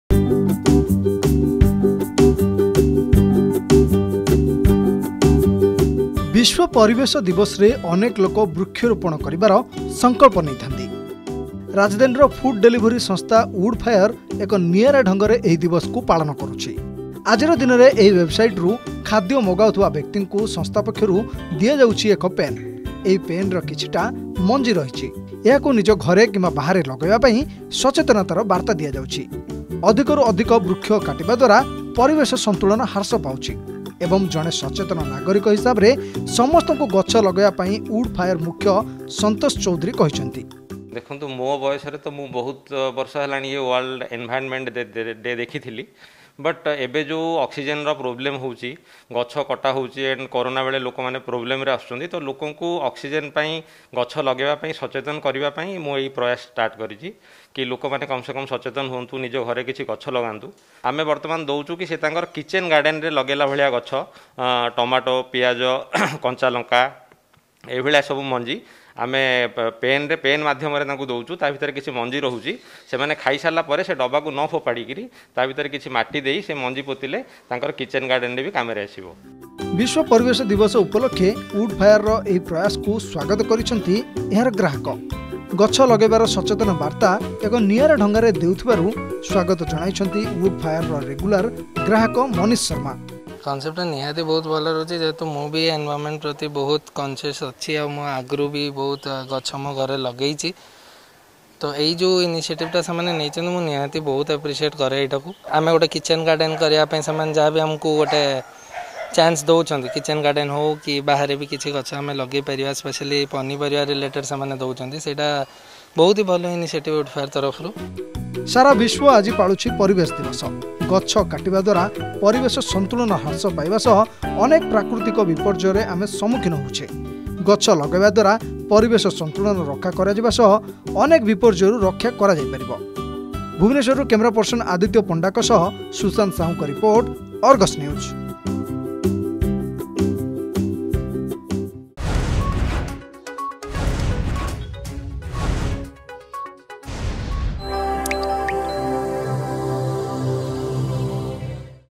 ଆଜି ହେଉଛି ଅନ୍ତର୍ଜାତୀୟ ପରିବେଶ ଦିବସ।
ଆଜି ହେଉଛି ଅନ୍ତର୍ଜାତୀୟ ପରିବେଶ ଦିବସ। ଏହି ଦିବସକୁ ନିଆରା ଢଙ୍ଗରେ ପାଳନ କରିଛନ୍ତି ଉଡ଼ ଫାୟାର ଅନଲାଇନ ଫୁଡ ଡେଲିଭରି ସଂସ୍ଥା। ରାଜଧାନୀର ଫୁଡ ଡେଲିଭରି ସଂସ୍ଥା ଉଡ଼ ଫାୟାର ଏକ ନିଆରା ଢଙ୍ଗରେ ଏହି ଦିବସକୁ ପାଳନ କରୁଛି । ଆସନ୍ତୁ ଦେଖିବା ଏହି ରିପୋର୍ଟ ।